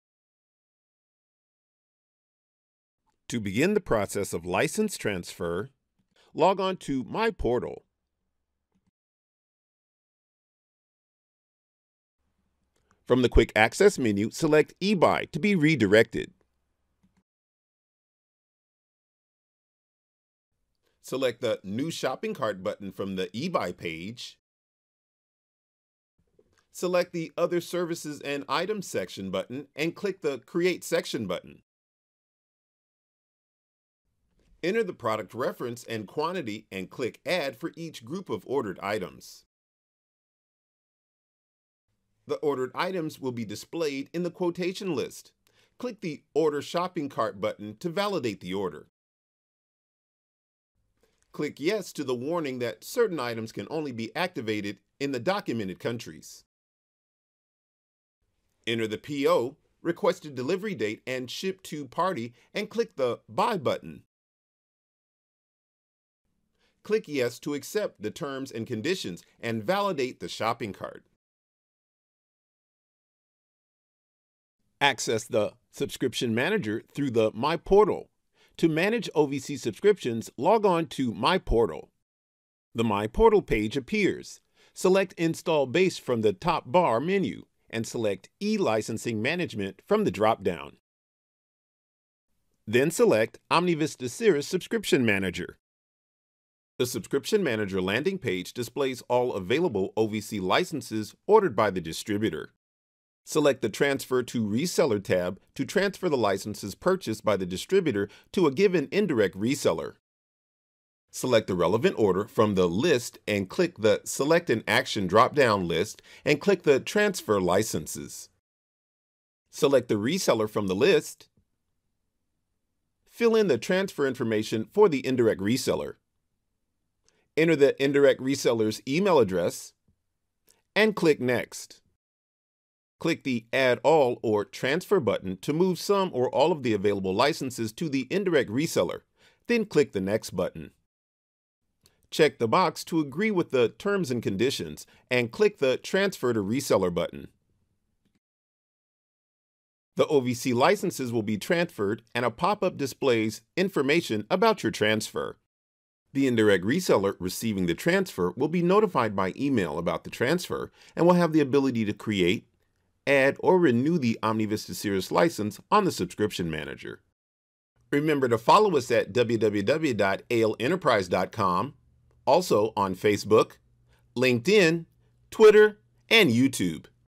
OmniVista Cirrus - License transfer from a Distributor to an Indirect Reseller
As a Distributor, learn how to transfer OmniVista Cirrus licenses to a given Indirect Reseller.